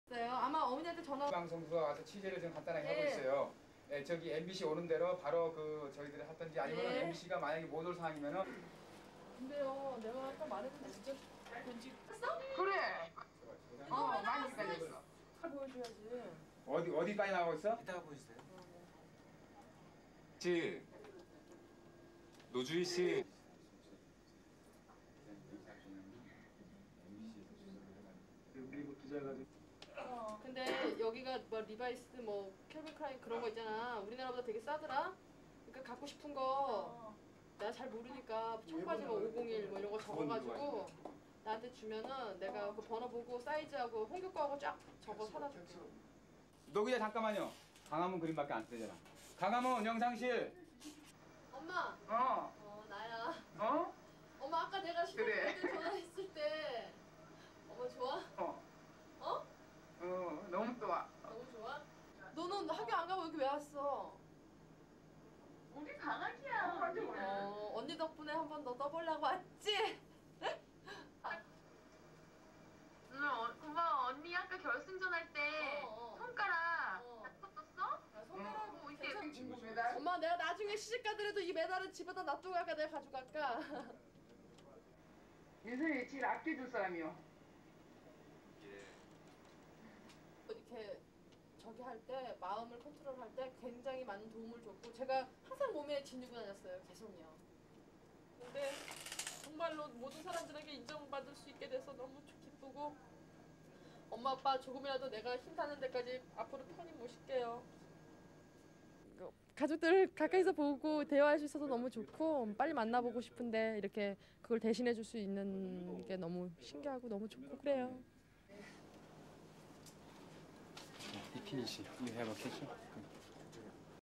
USA: OLYMPICS 96: SOUTH KOREAN JUDO MEDALIST PHONES HOME
(23 Jul 1996) Korean/Nat

Satellite technology given free to a South Korean athlete brought out emotional scenes in Atlanta Monday, as middle weight judo champion Cho Min-Sun celebrated her gold medal at the Olympic Games.

During a two-way TV call with her mother and sister back home, the gold medalist could not contain her emotions, and burst into tears of joy.

It was all smiles for Cho Min-Sun as she celebrated her third international middleweight (66 kgs) win in judo.

Twice world champion - in 1993 and -95, Cho clinched Olympic gold in her class Monday, beating off her Polish opponent.

And she was able to share the glory with absent family members, thanks to Korea Telecom, who reward any Korean athlete who wins with a two-way satellite conversation back home.

Chu spoke at length with her mother and sister, proudly waving her medal at them.

Emotions ran high, and the champion could eventually not contain herself.

SOUNDBITE:(Korean)
(Q: How does it feel to contact your mother and sister using satellite technology?)

"It's great and it's a wonderful feeling to be able to see them and for them to see me and my medal. But it's not the same as being back home with them."

SUPER CAPTION: Cho Min-Sun, Middle Weight Judo Gold Medalist

Following the games, 24-year-old Cho will return to Seoul, where she is studying Physical Education at the Korean National University.